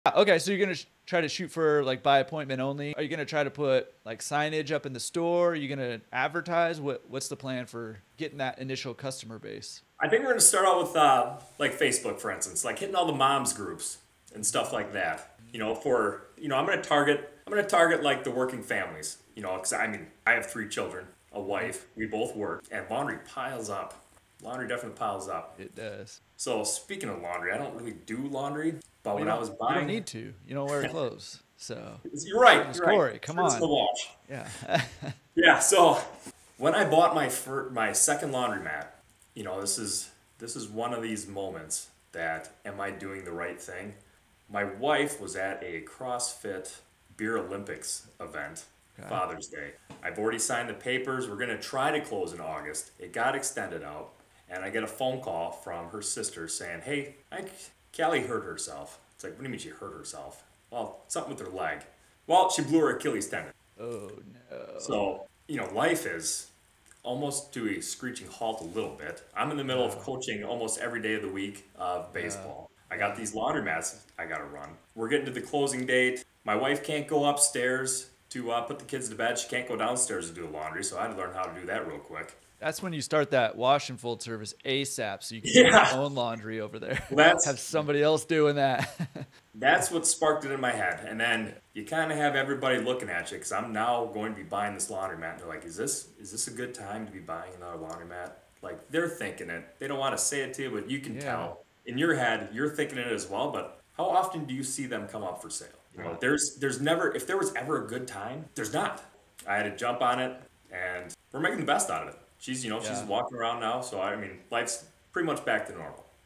The SECRET To Getting Your FIRST 100 Customers for Your Laundromat
Are you a laundromat owner looking for new customers? Don't worry, we have the perfect solution for you! Join us as we discuss the best strategies to get your initial customer base up and running. We'll show you how to start advertising on Facebook, and how to reach groups of people who could benefit from your service. Learn how to tap into mom groups, working families, and more - giving your business the boost it needs! Tune in now for all the essential tips and tricks!